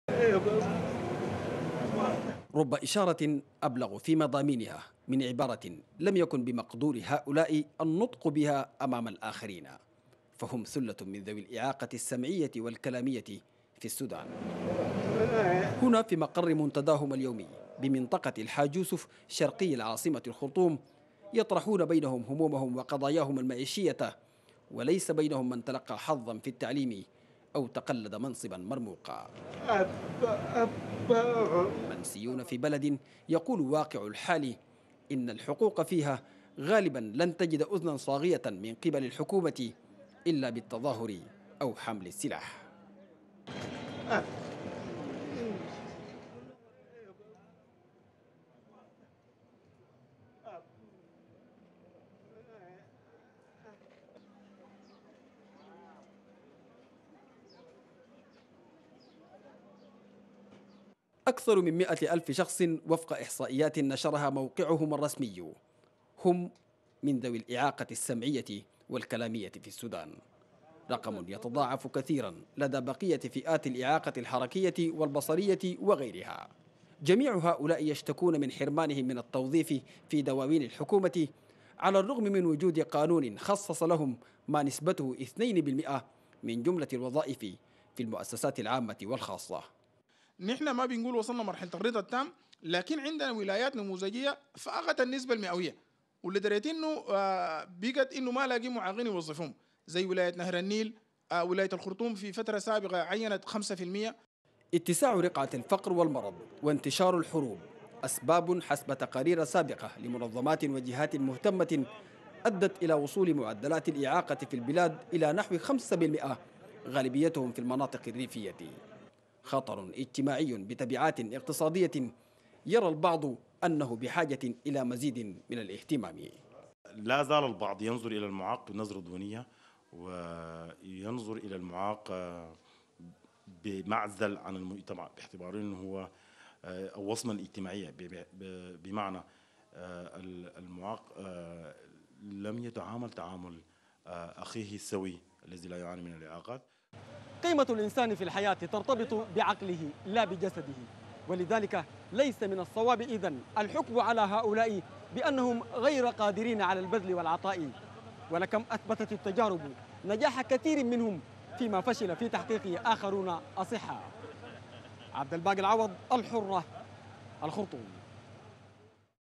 فئة الصم والبكم تعاني من قلة مناصب العمل والافتقار إلى المراكز التعليمية في السودان
يبلغ عدد ذوي الإعاقةِ السمعية والكلامية في السودان أكثر من  مئة الف شخصٍ. خمسة وستون في المئة منهم أميون بسبب عدم توفر مراكز  التعليم الخاصة بهم وقلة المعلمين الذين يتقنون لغة الإشارة.